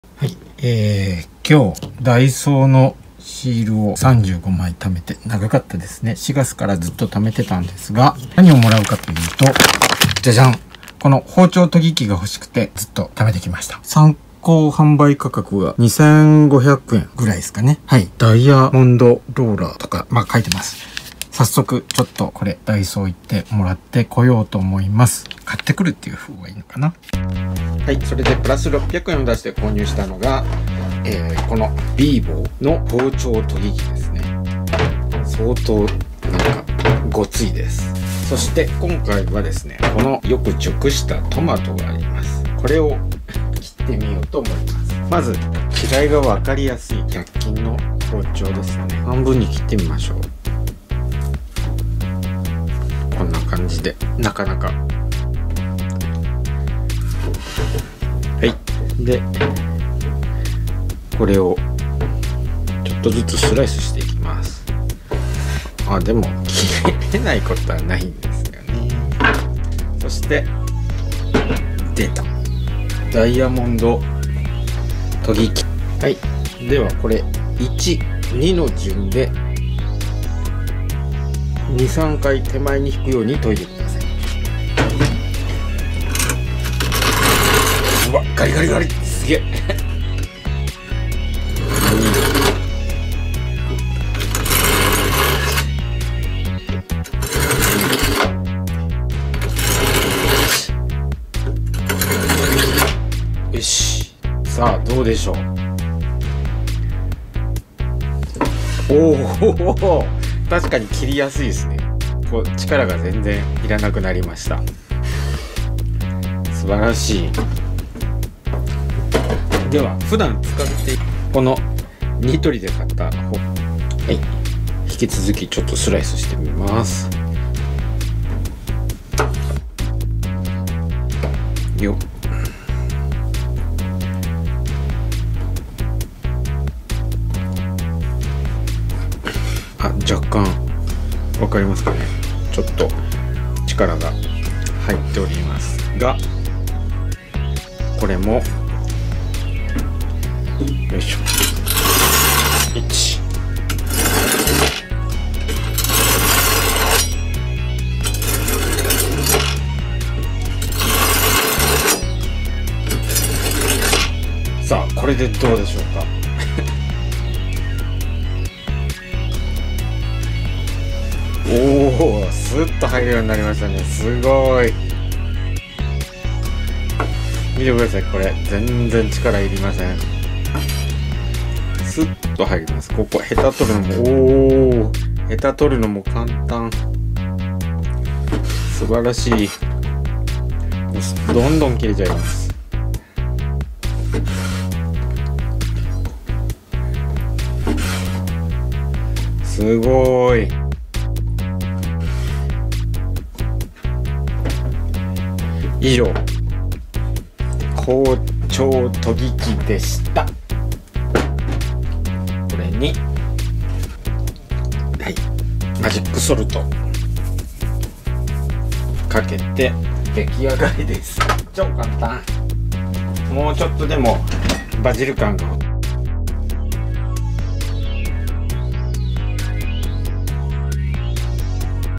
ダイソーのシールを約3ヶ月で35個集めてVIVOの包丁研ぎ器を手に入れてみた
300円(税込み）購入ごとに1枚もらえるダイソーのシールを約3ヶ月で35個集めてVIVOの包丁研ぎ器を手に入れてみました。切れ味悪い包丁がどこまで切れるようになるのか。実験。